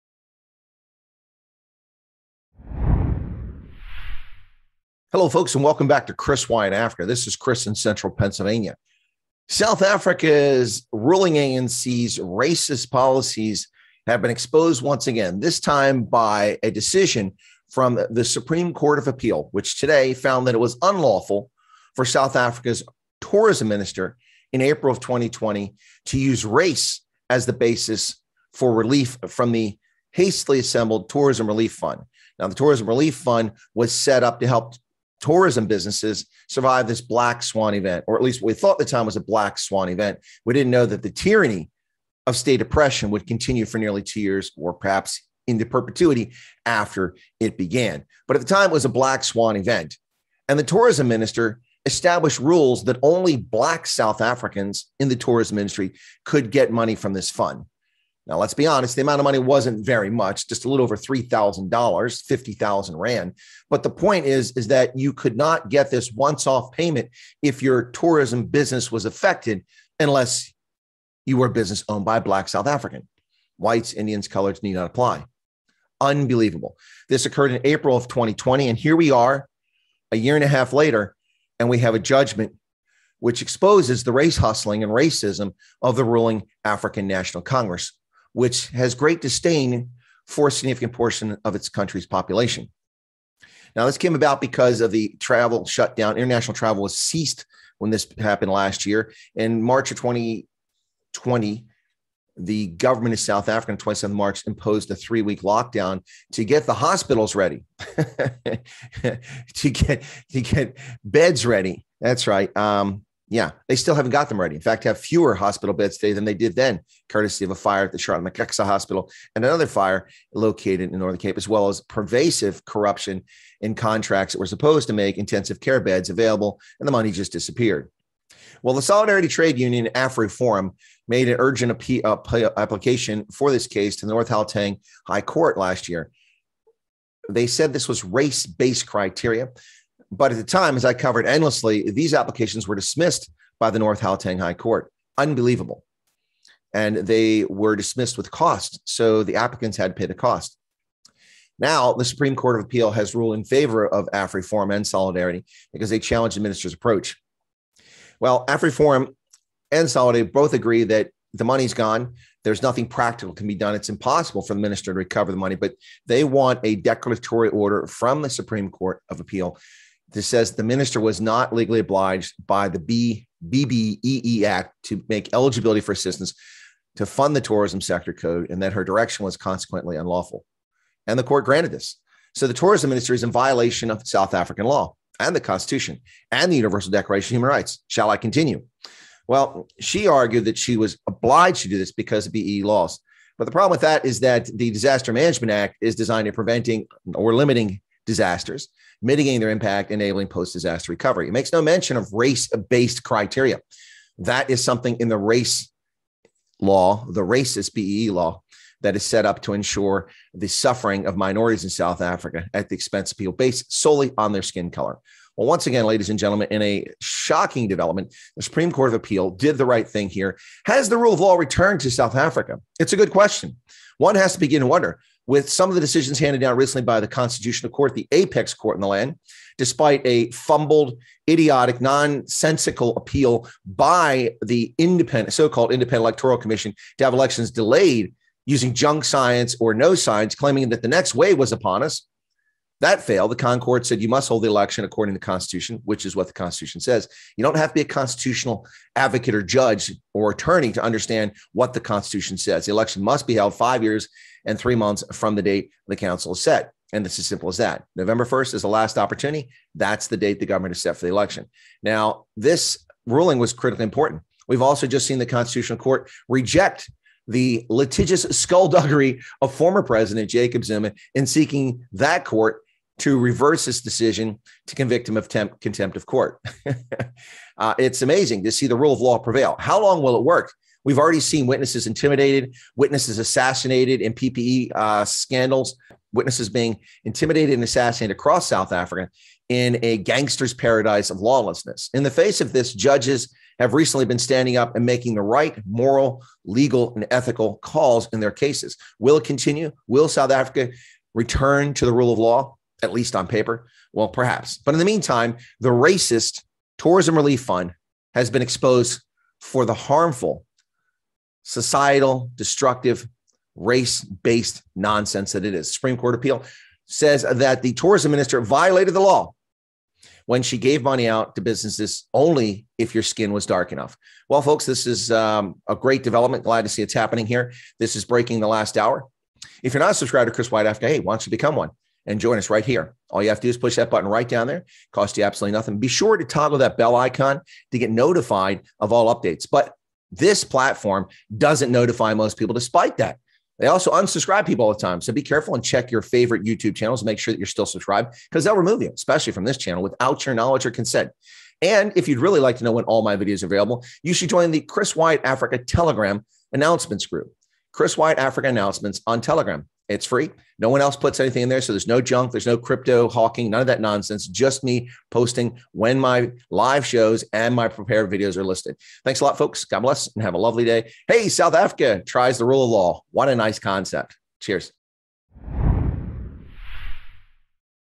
South African Court Declares Minister Ngubane's Racist Tourism Relief Fund Unlawful
SouthAfrica, pandemic, tourism, ANC, racism, AfriForum, Solidarity Trade Union, Mmamoloko, Kubayi-Ngubane

Animation credit: Talon Bezuidenhout
Opening news drums sequence: Kevin Nicklin